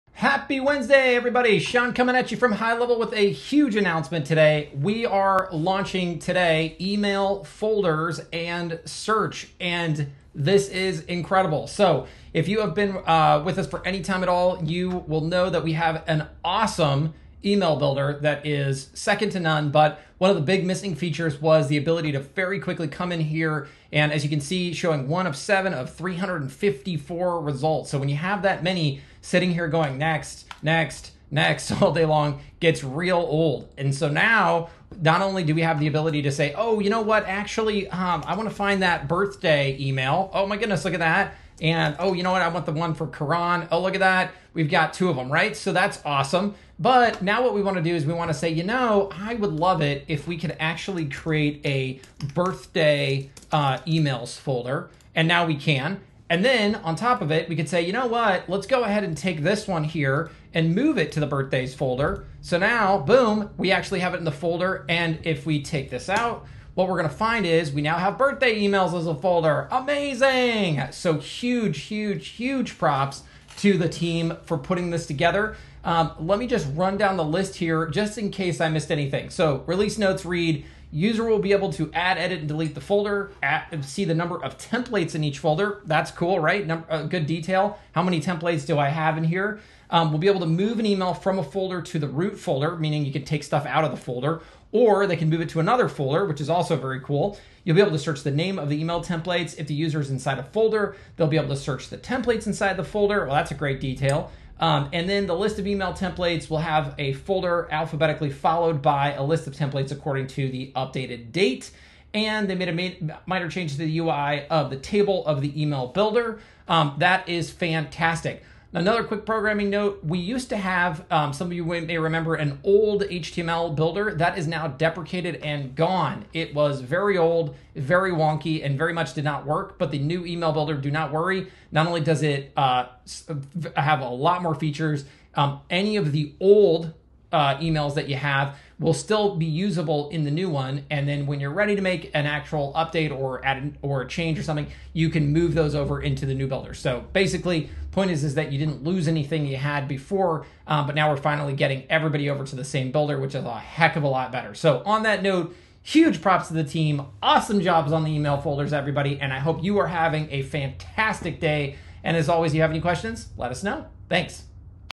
Email Folders + Search Live! Woohoo!
You can now search for email templates and organize them in folders!